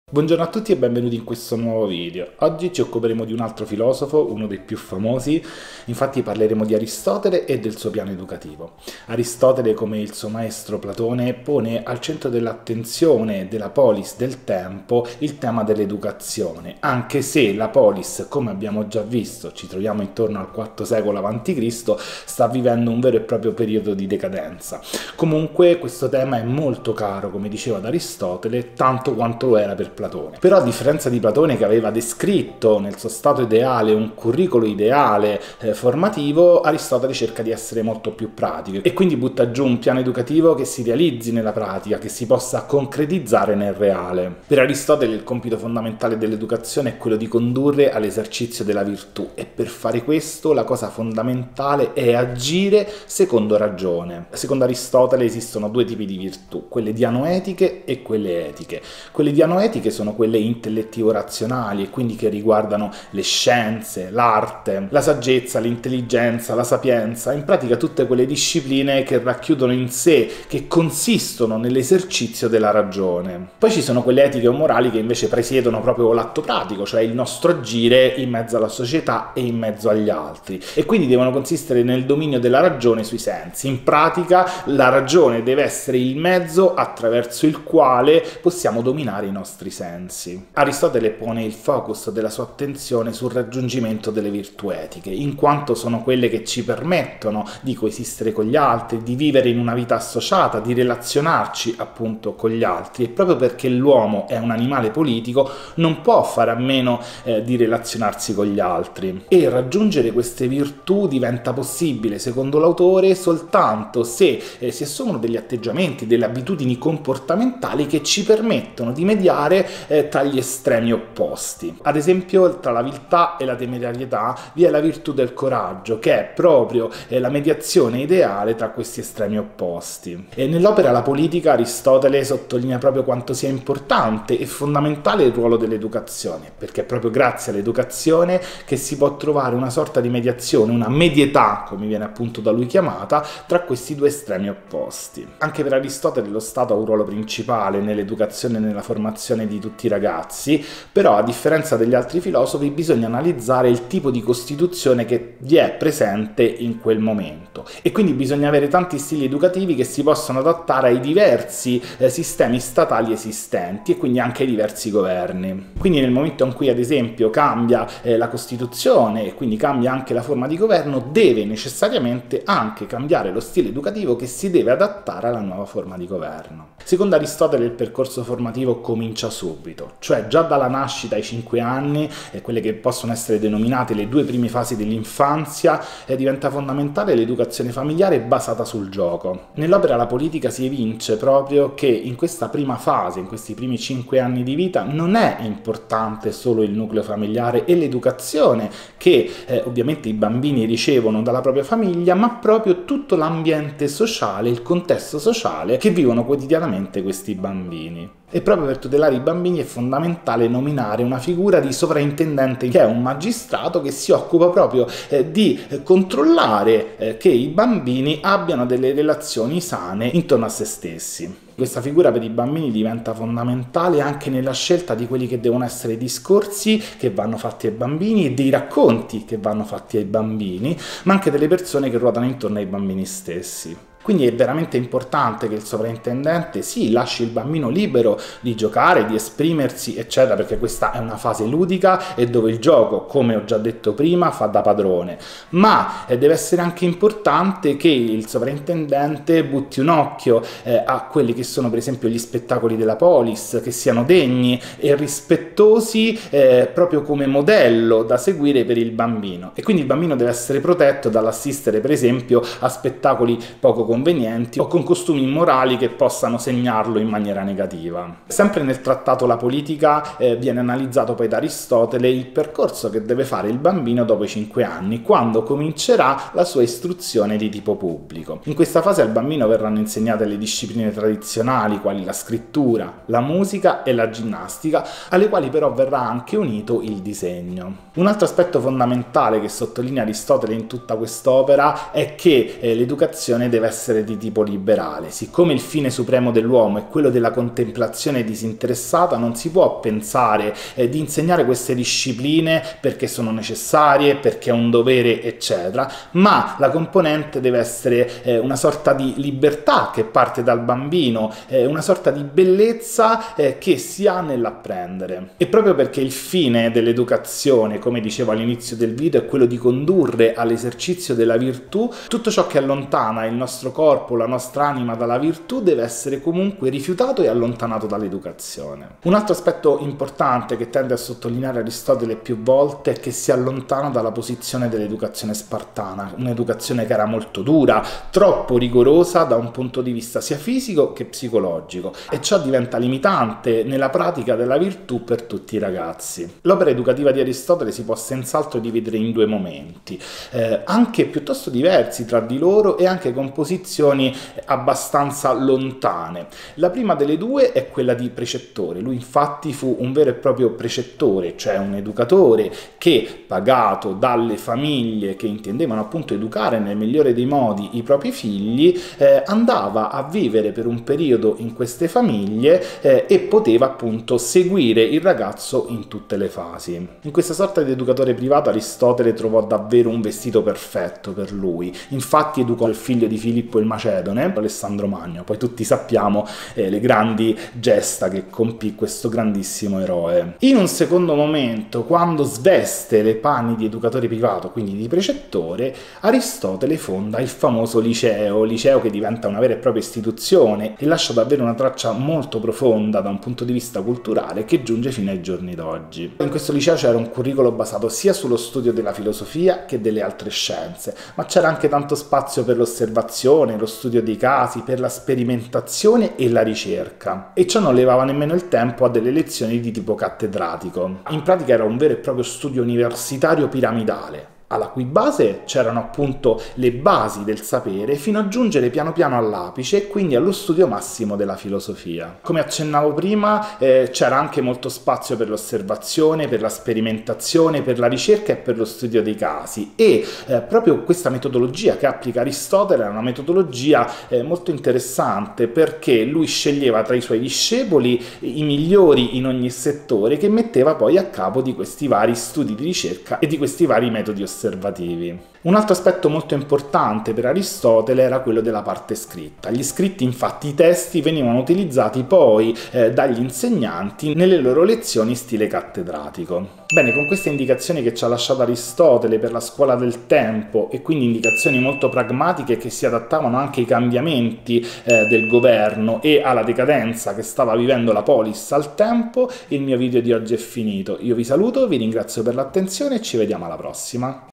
L'educazione per Aristotele
in questo video si parla di Aristotele, della sua visione pratica dell'educazione finalizzata all'esercizio della virtù, del compito dello Stato di educare in base alle esigenze della Costituzione, del percorso formativo sotto l'occhio attento di un sovraintendente all'infanzia, dell'educazione pubblica nelle varie fasi e del ruolo che egli stesso ha avuto di precettore prima e fondatore del liceo dopo.
Il video nasce per i ragazzi del liceo delle scienze umane, ma si presta alla visione di tutti gli appassionati del settore, e alla preparazione per il concorso scuola.
Per qualsiasi richiesta di approfondimenti, consulenze private e lezioni personalizzate potete scrivermi qui: